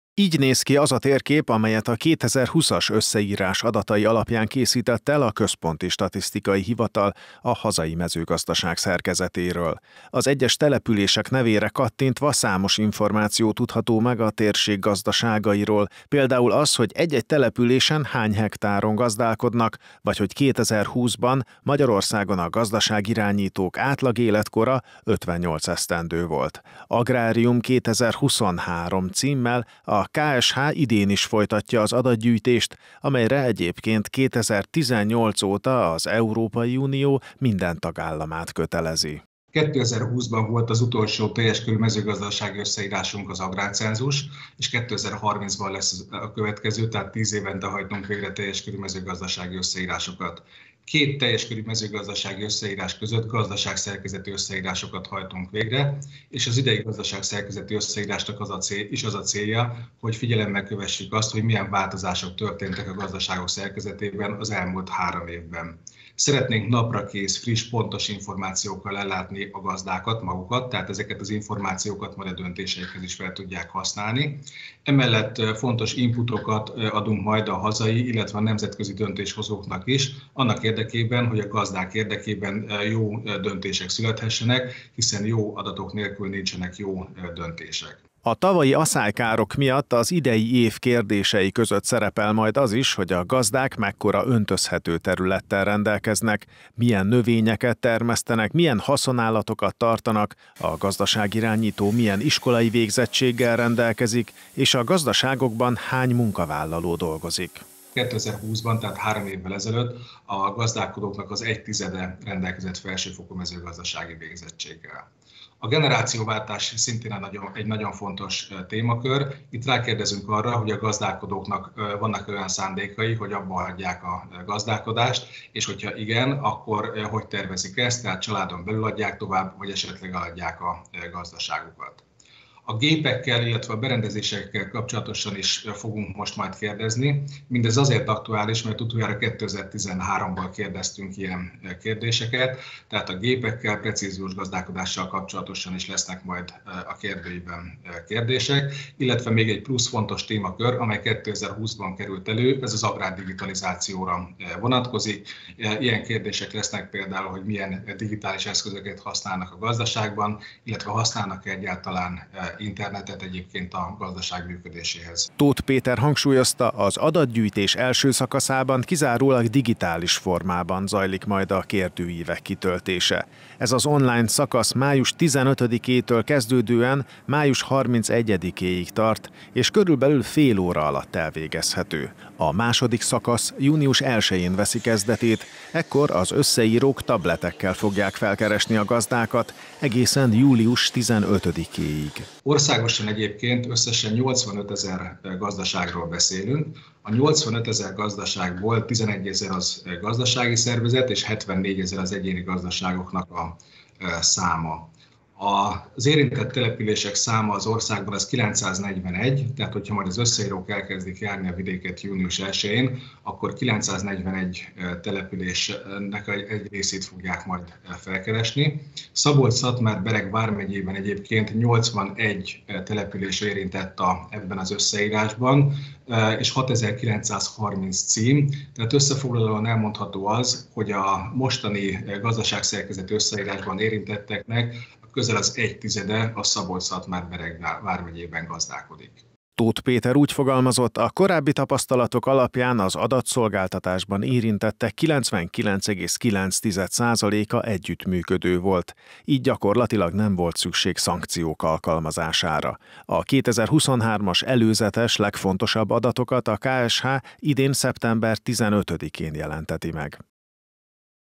Mezőgazdasági összeírás indul
Gazdaságszerkezeti összeírást indít a Központi Statisztikai Hivatal május 15. és július 15. között. A mezőgazdasági termelőket keresik meg, akik online kérdőívek kitöltésével tudnak majd számot adni gazdaságukról. Akik az első szakaszban nem élnek az online válaszadás lehetőségével, azokat a KSH munkatársai személyesen fogják felkeresni.